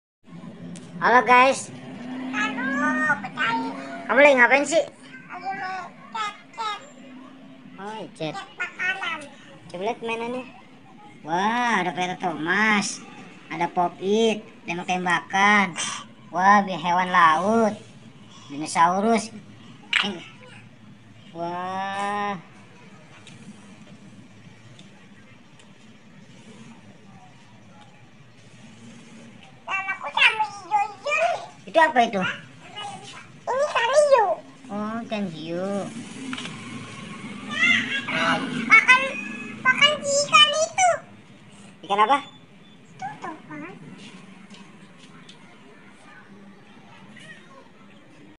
Viral Membersihkan Mainan Tembakan, Kereta Thomas, Pop it, Puzzle, Hewan Laut
Viral Membersihkan Mainan Tembakan, Kereta Thomas, Pop it, Puzzle, Hewan Laut seru banget.

Adam lagi asik banget bersihin mainanya, Sampai badanya basah.